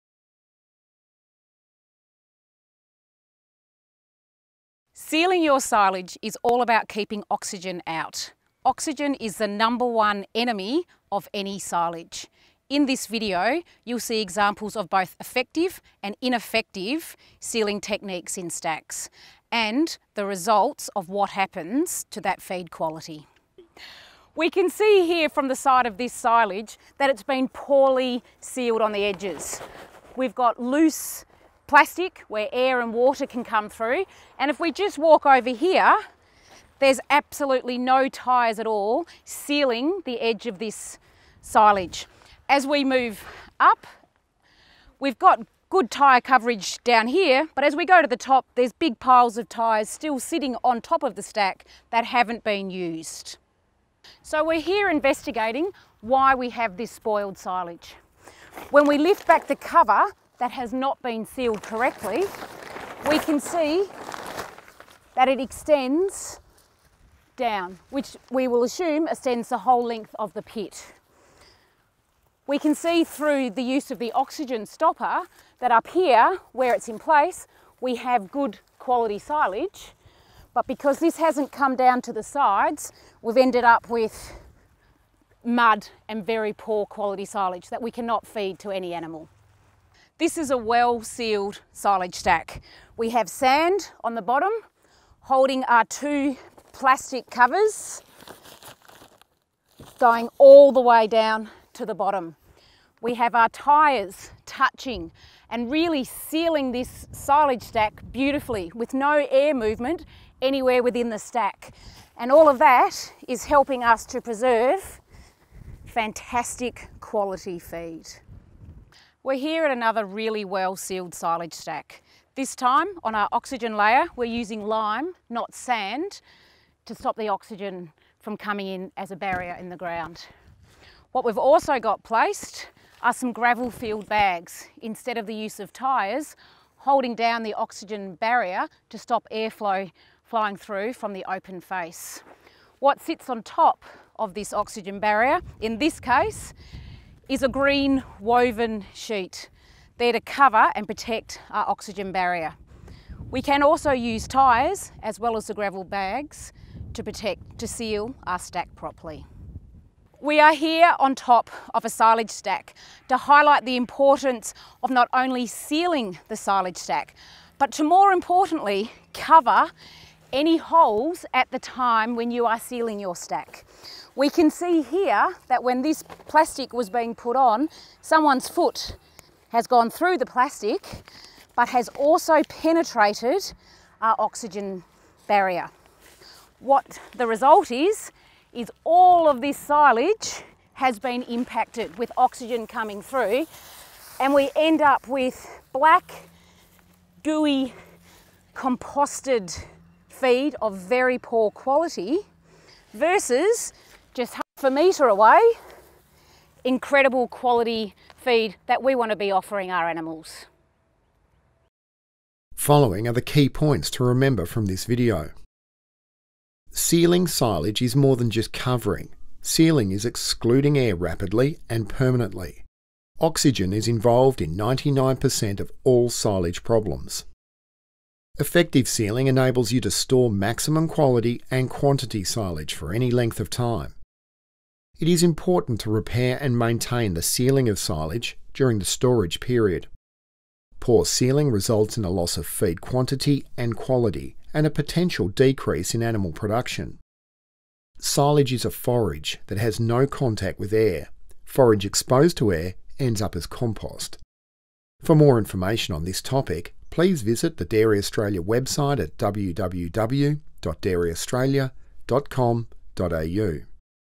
Successful Corn Silage - Sealing
Showing examples of different silage seals and sealing techniques, Murray Dairy's Lisa Birrell demonstrates the importance of a good seal to maintain silage quality.